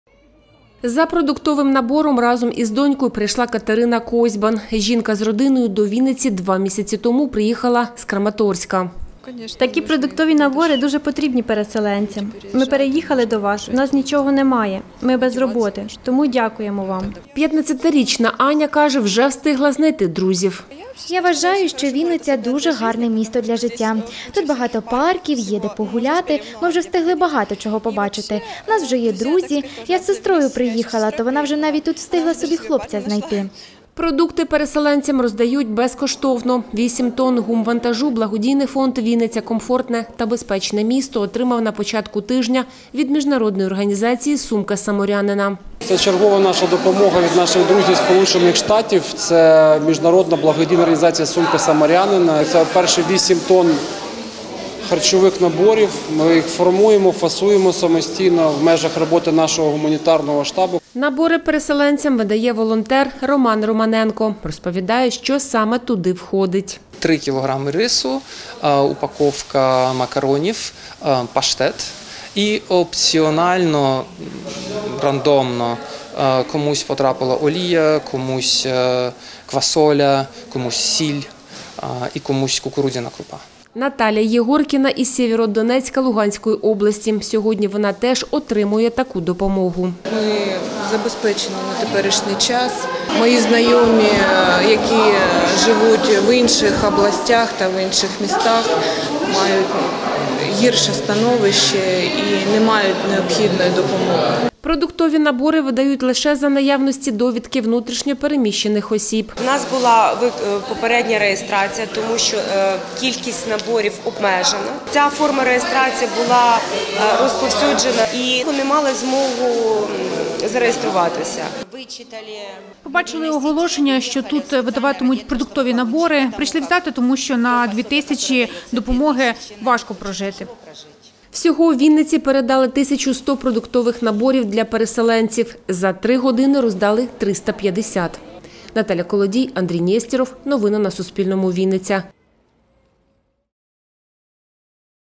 "Сумка саморянина". У Вінниці переселенці отримали понад тисячу продуктових наборів
Крупи, олію, макаронні вироби, паштети — загалом, 1100 продуктових наборів для переселенців у Вінницю передала благодійна організація «Сумка саморянина» зі США. Про це Суспільному розповів керівник фонду “Вінниця — комфортне та безпечне місто» Андрій Ковальов. Допомога надійшла на початку тижня. Інформацію про це виклали у соцмережах. Там переселенці заповнювали відповідну анкету і вже сьогодні з необхідними документи прийшли по допомогу.